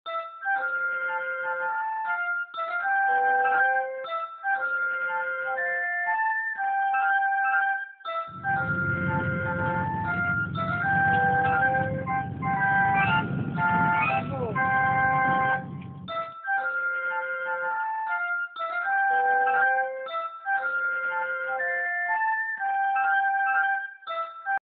steve waving on a train
on the way back from a day out ice skating on a train